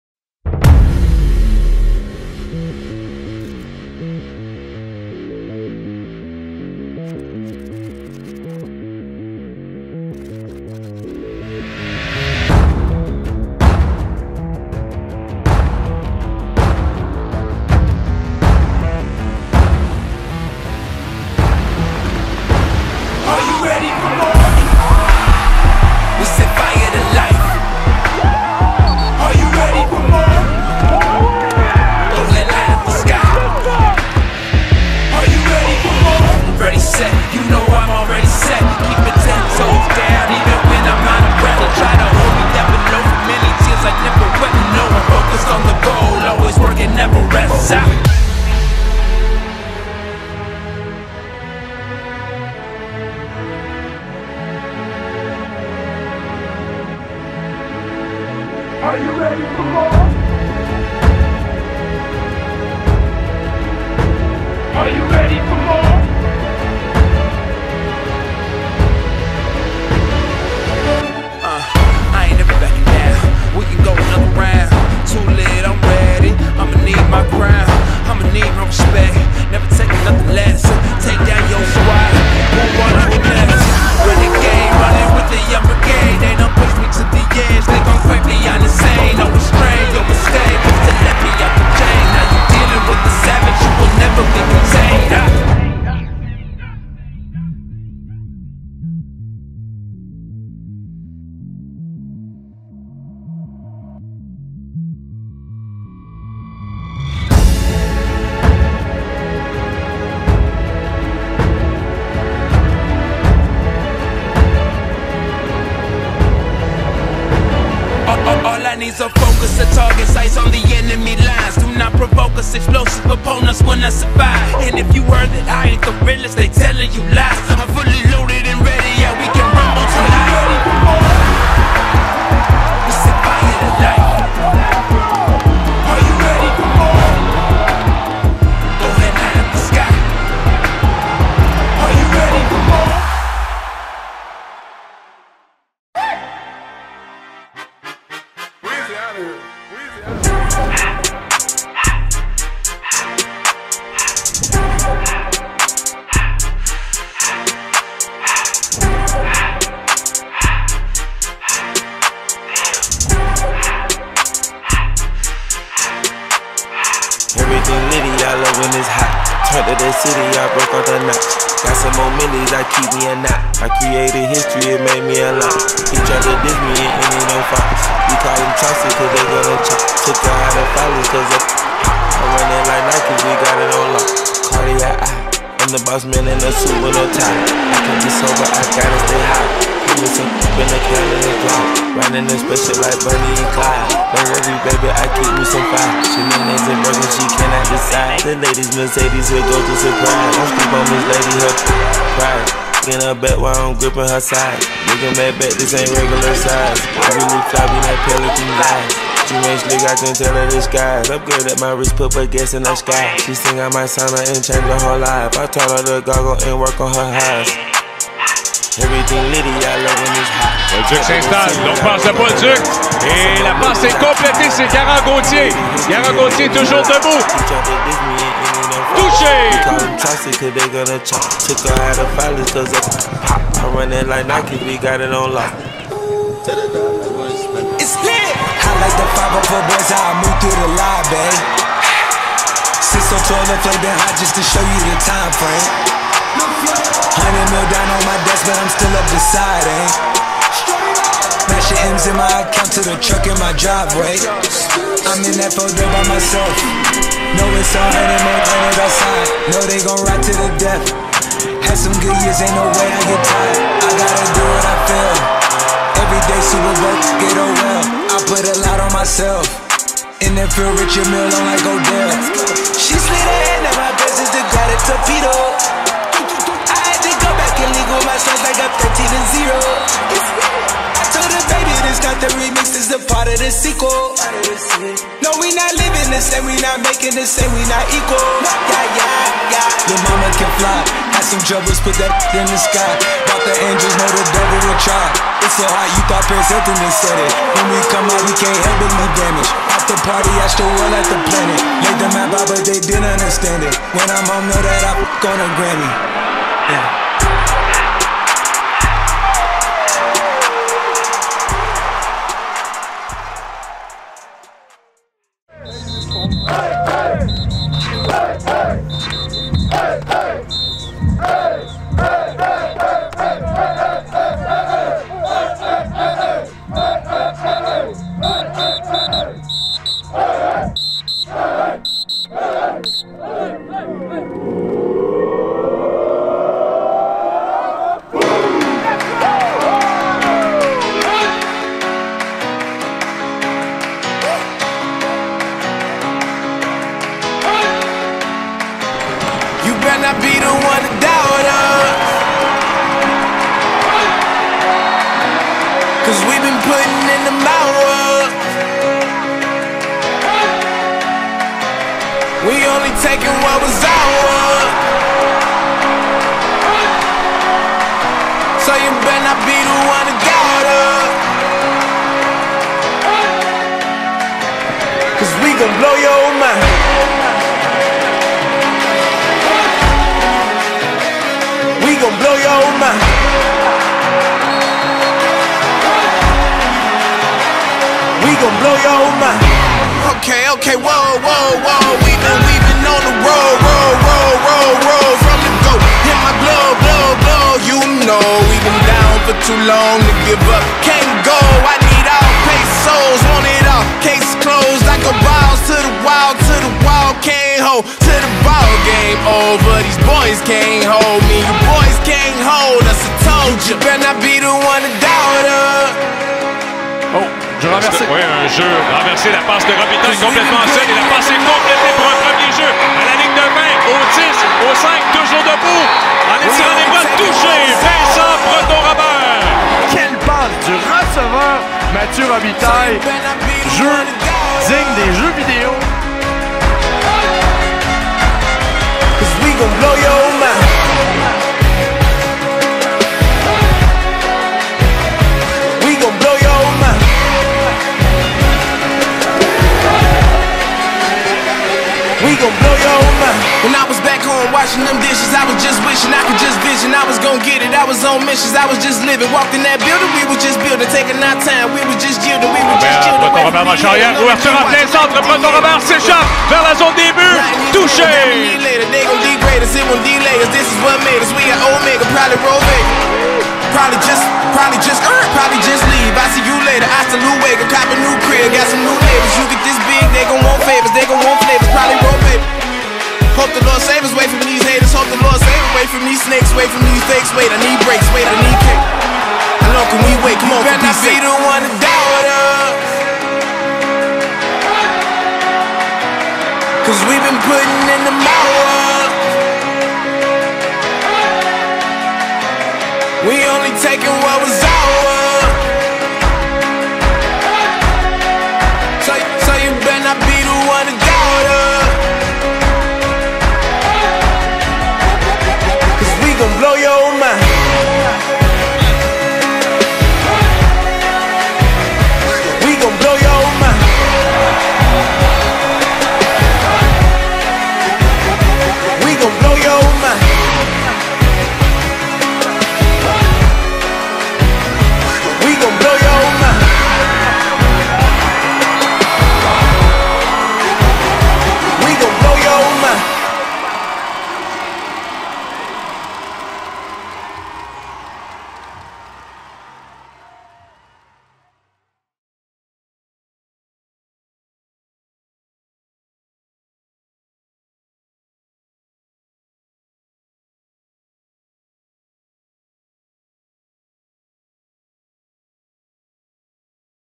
Rouge et Or Football Highlight 2019
Highlight de la saison 2019 du Rouge et Or football de l'Université Laval.